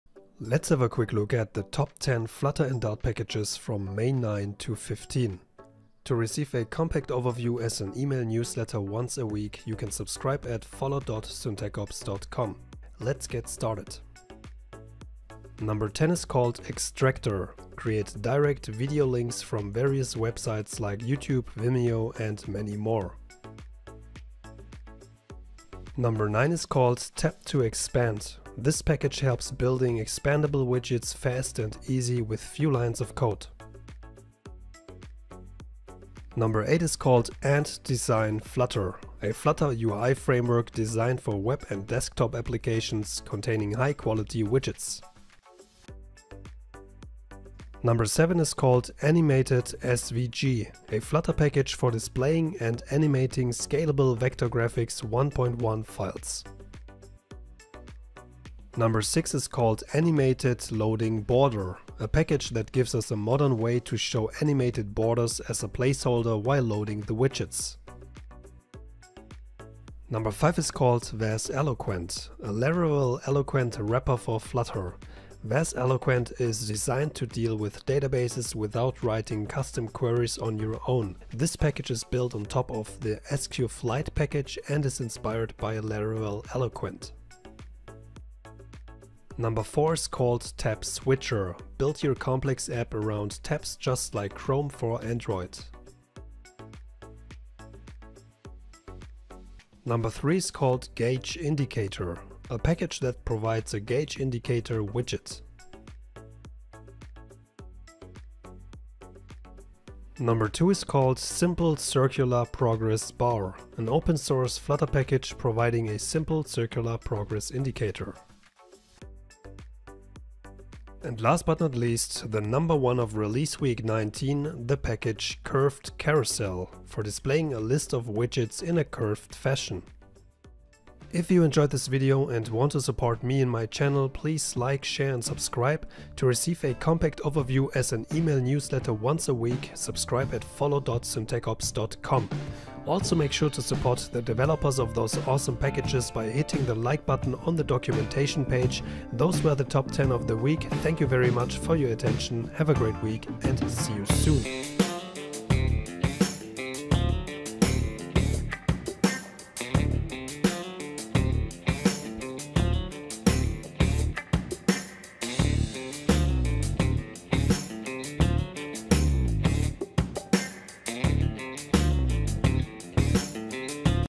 Flutter Curved Carousel, Gauge Widget & Co. - 19 - PUB.DEV RELEASES 2022
Week 19 brings a curved carousel widget for Flutter  and many more useful packages.

🕒 TIMESTAMPS:
00:00 - start
00:05 - newsletter
00:15 - extractor
00:25 - tap_to_expand
00:35 - ant_design_flutter
00:47 - animated_svg
00:58 - animated_loading_border
01:09 - wazeloquent
01:28 - tab_switcher
01:39 - gauge_indicator
01:49 - simple_circular_progress_bar
02:00 - curved_carousel
02:10 - closing